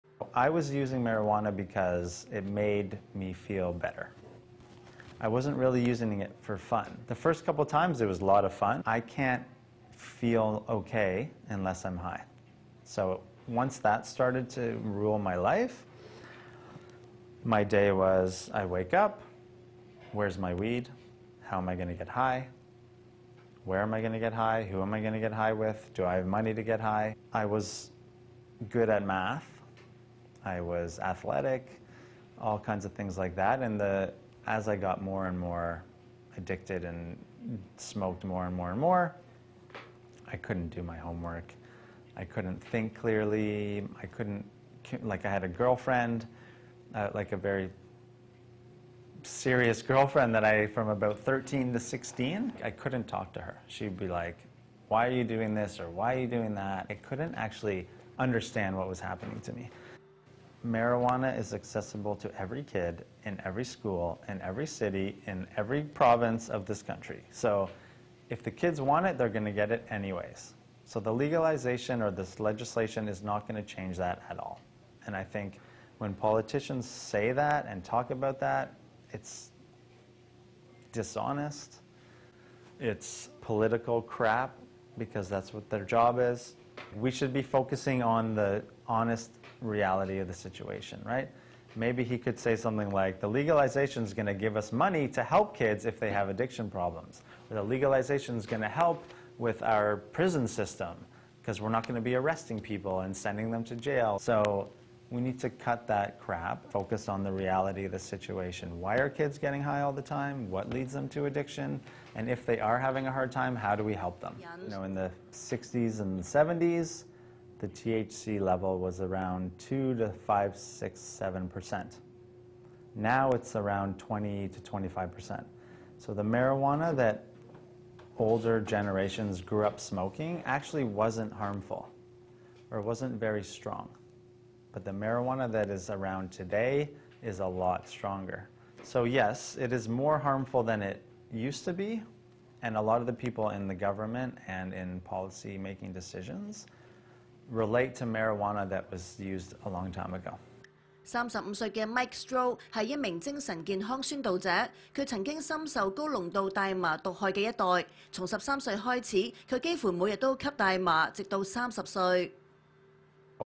Is WEED Legalization BAD for SOCIETY?
We desperately need more sincere and honest dialogue in society! Here's my attempt to call out a little hypocrisy.

Politicians often say they care about young people or they're out there to protect them. It's usually political nonsense and grandstanding. The best thing for us to do is be honest and talk about the real situations that are already happening in the lives of young people.

Peace